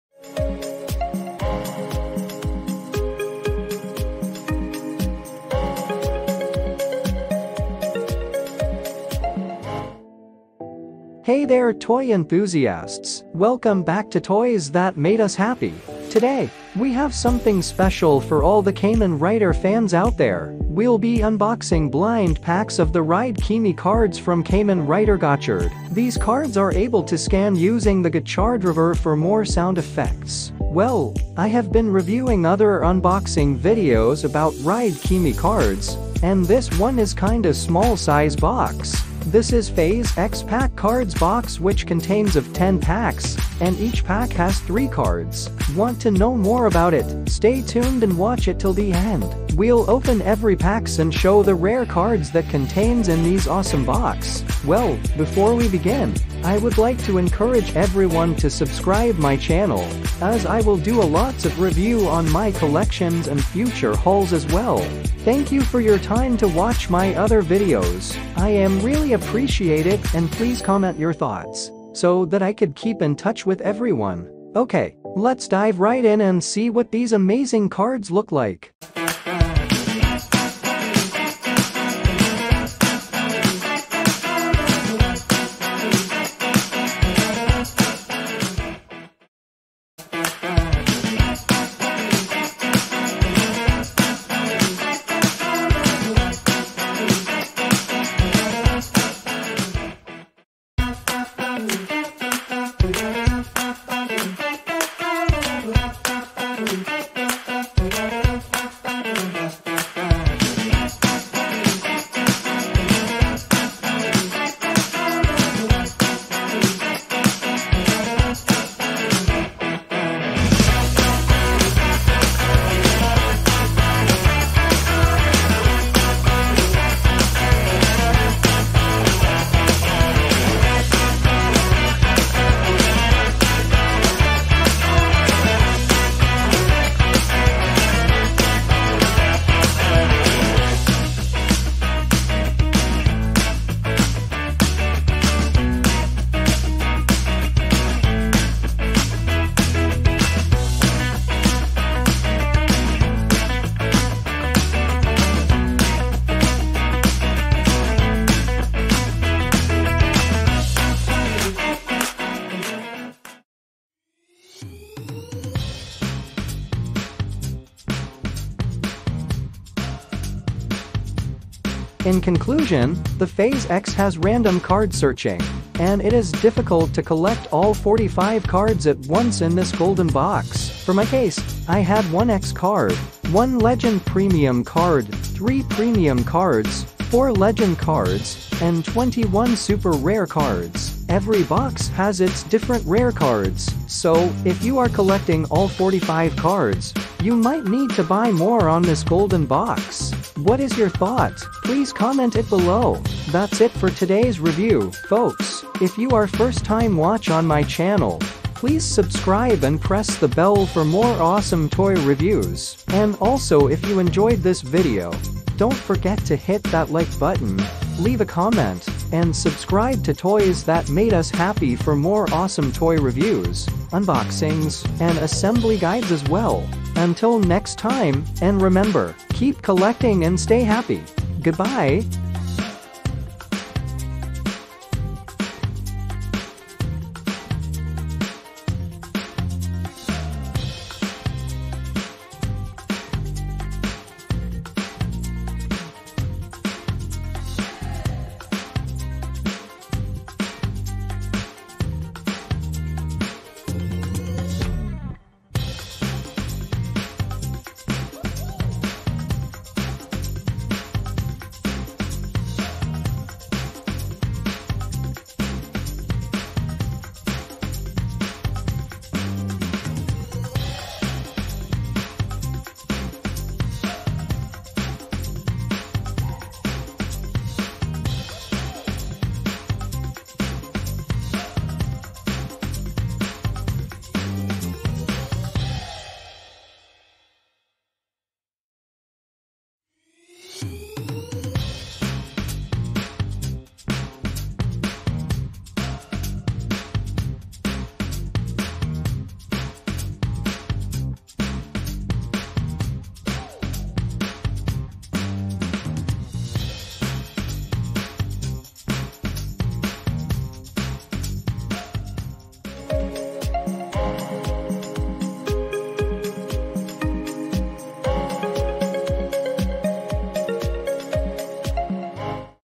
Bandai DX Rider Chemy card Phase:EX Kamen rider Gotchard review
Dive into the world of Kamen Rider Gotchard with our in-depth review of the Bandai DX Rider Chemy Card Phase: EX! Join us as we unbox and explore every detail of these collectible cards, showcasing their design, features, and compatibility with the Kamen Rider Gotchard series. Perfect for fans and collectors, this review highlights the excitement and nostalgia of the Kamen Rider universe. Don't miss out on this must-watch review to enhance your collection!